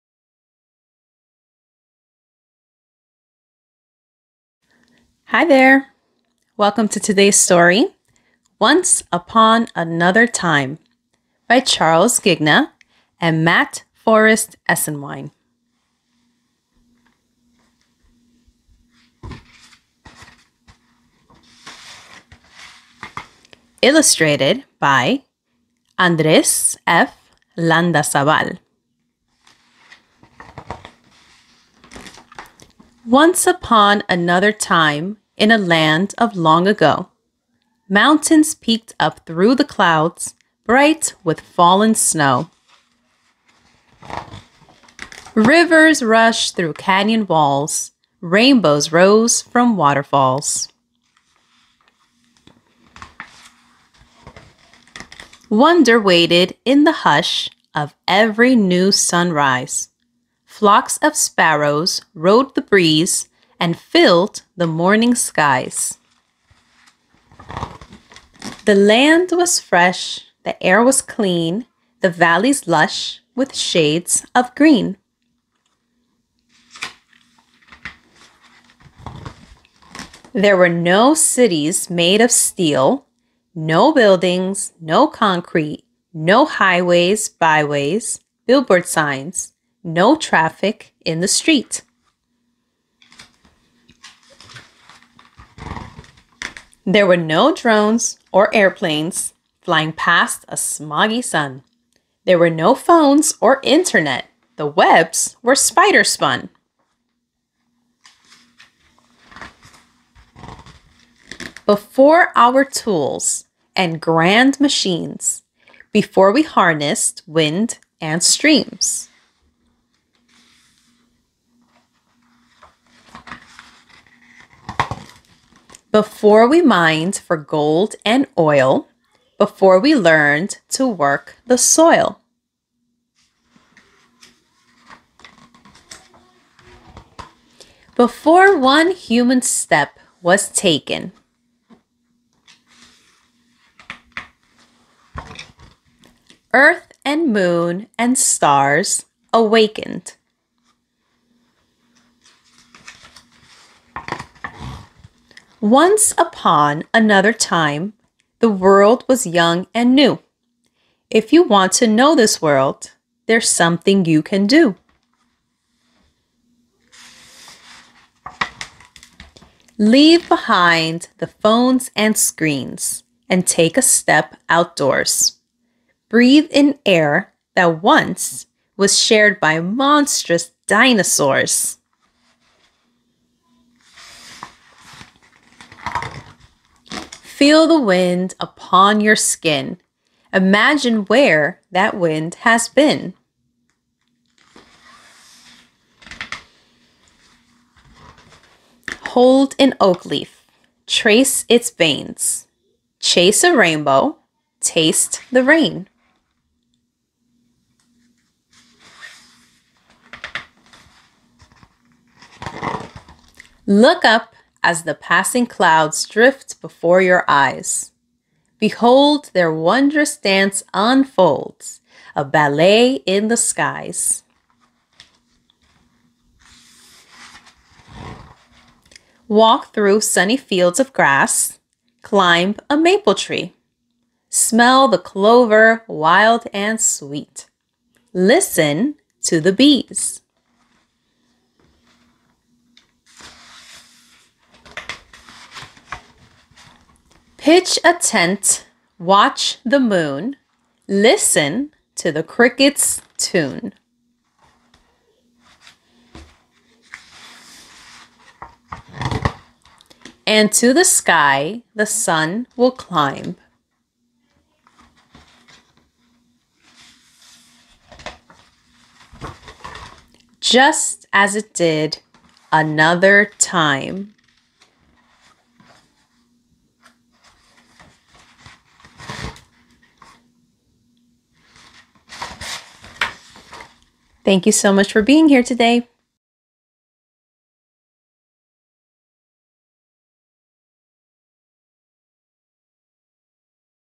Once Upon Another Time Read Aloud by Ms. Yes [ Short Story ]
Ms. Yes reads Once Upon Another Time by Charles Ghigna and Matt Forrest Esenwine, Illustrated by Andres F. Landazabal

“Once upon another time, the world was young and new. If you want to know this world, there's something you can do...With sweeping landscapes and up-close details of the natural world, Once Upon Another Time takes readers through a lyrical exploration of the world as it was before humans made their mark. Contrasting the past with the present, this expansive picture book serves as a warm invitation for children--and all people--to appreciate, explore, and protect the magic and wonder of this planet we call home.” - Thriftbooks